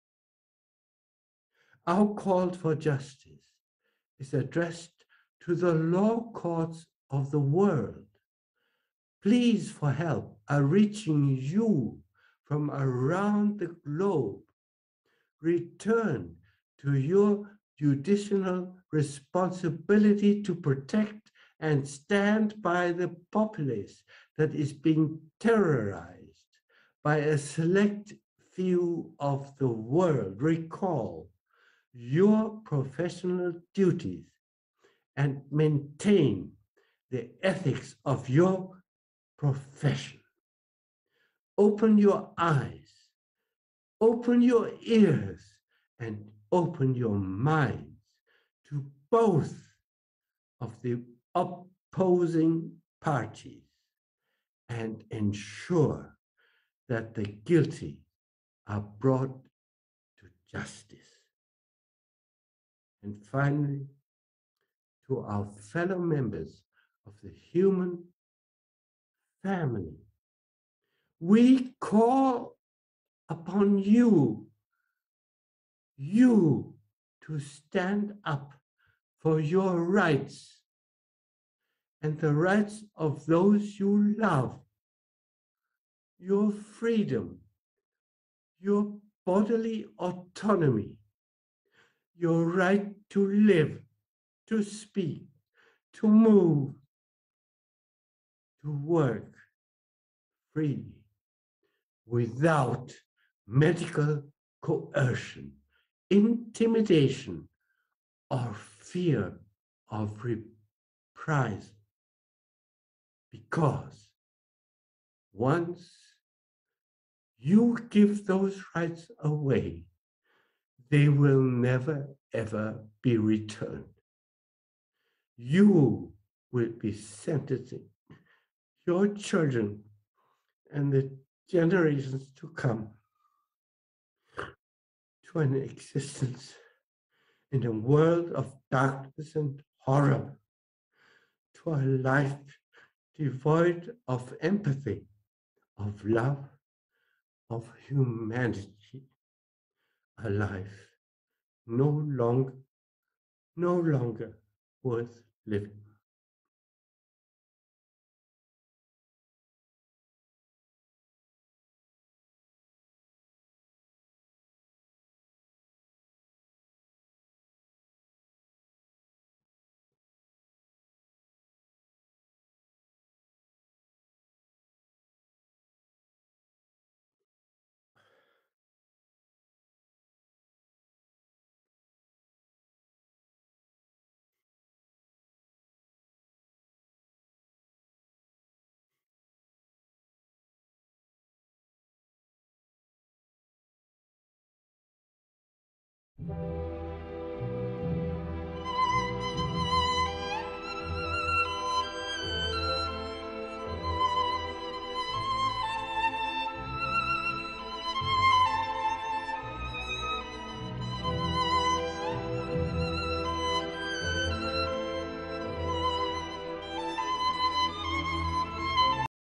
Dr. Sucharit Bhakdi - A World Of Darkness And Horror Is Being Formed
Source:
Doctors for Covid Ethics: An Interdisciplinary Symposium II - Sounding the Call